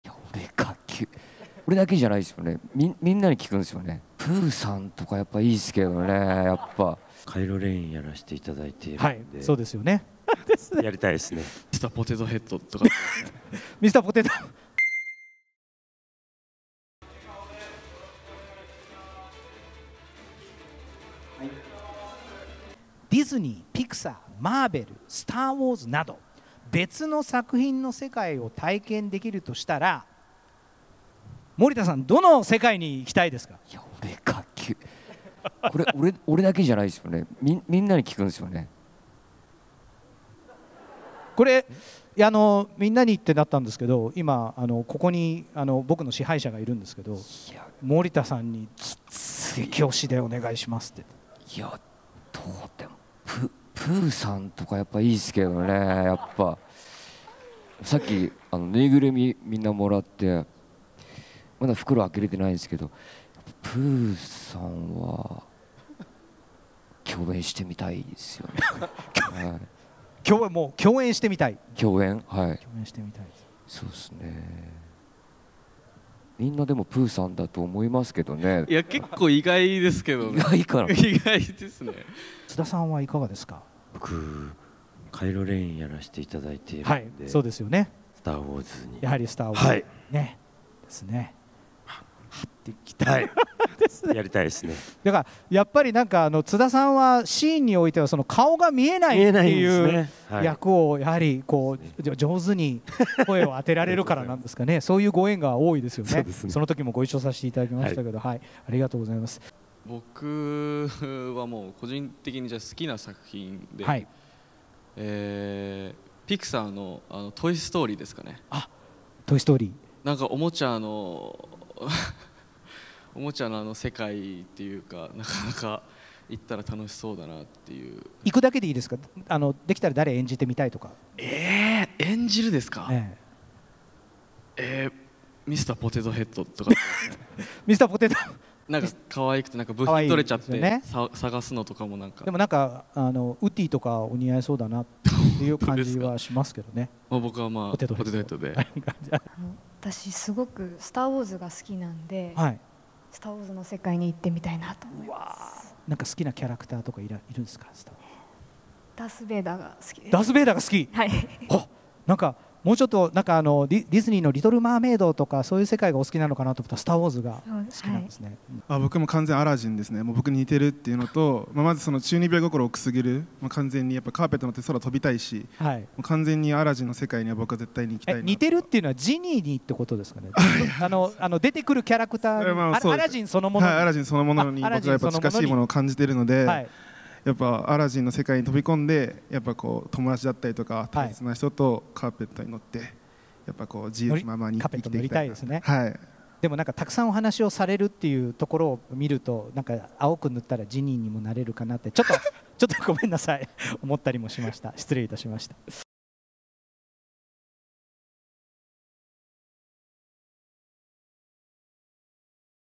津田健次郎、吹き替え務めた『スター・ウォーズ』の世界に憧れ 森田剛は「プーさんと共演したい」個性的な回答続出に会場爆笑『ワンダーハッチ -空飛ぶ竜の島-』配信記念ワールドプレミアイベント
『ワンダーハッチ -空飛ぶ竜の島-』 配信記念ワールドプレミアイベントが11日、都内で行われ、出演と声優の中島セナ、奥平大兼、エマニエル由人、津田健次郎（声優のみ）、森田剛と萩原健太郎監督が登場した。